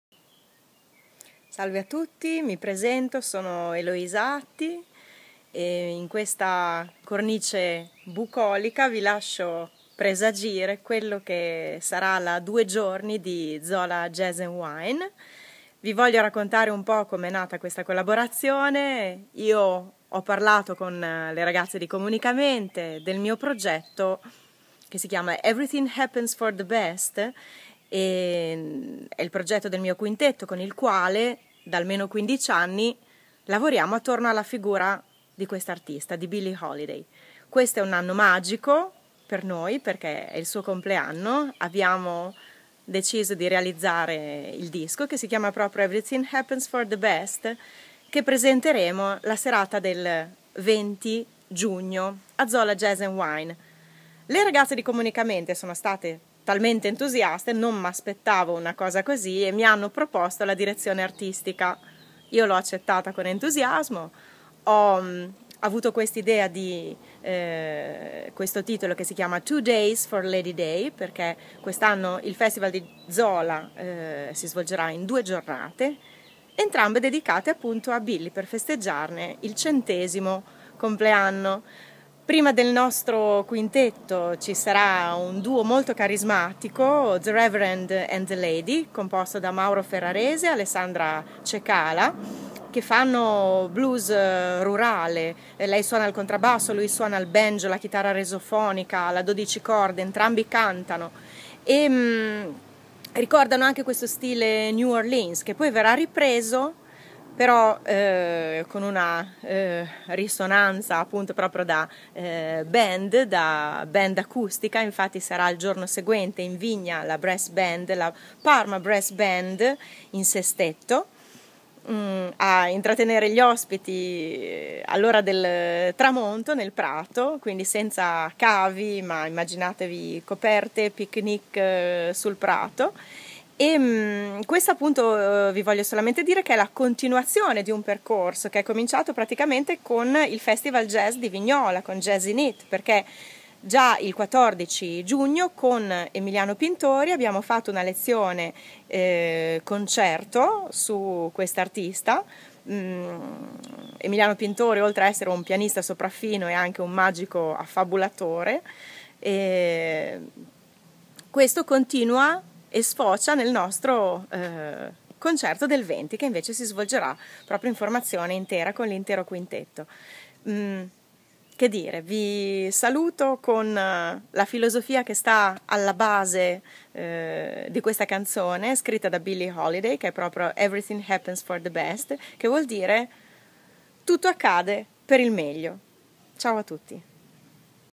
Presentazione di Zola Jazz&Wine 2015
La nostra direttrice artistica Eloisa Atti , in una cornice bucolica, ci racconta le sorprese che ha in serbo per Zola Jazz&Wine il 20 e 21 Giugno 2015!
Buon ascolto